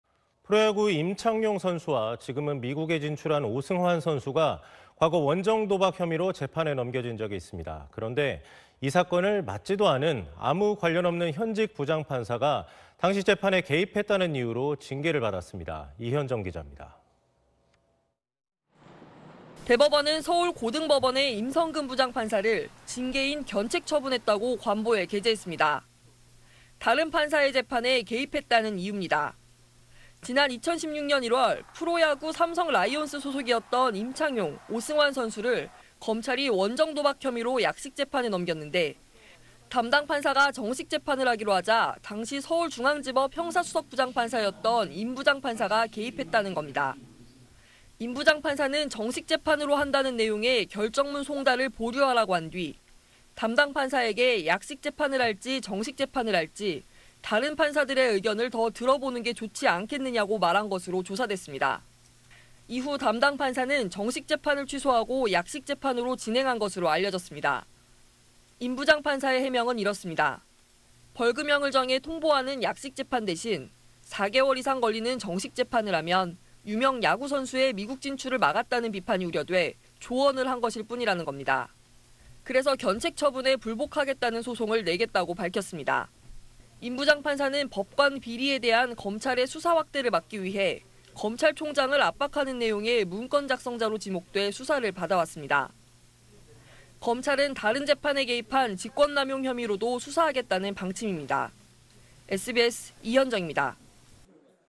'야구선수 도박' 재판 개입 법관 징계…"불복 소송 낼 것" / SBS
프로야구 임창용 선수와 지금은 미국에 진출한 오승환 선수가 과거 원정도박 혐의로 재판에 넘겨진 적이 있습니다. 그런데 이 사건을 맡지도 않은 아무 관련없는 현직 부장판사가 당시 재판에 개입했다는 이유로 징계를 받았습니다.

모바일: 'SBS 뉴스' 앱 설치하고 제보